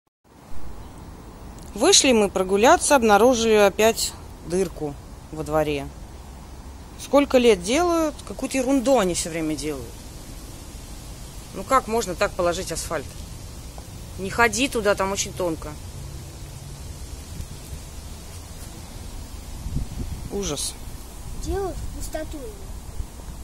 Это каждый год такое издевательство...
Каждый год заделывают одну и туже дырку и она через год опять появляется, неужели ее не заделать так, чтобы не было провала??? А слой асфальта просто умиляет...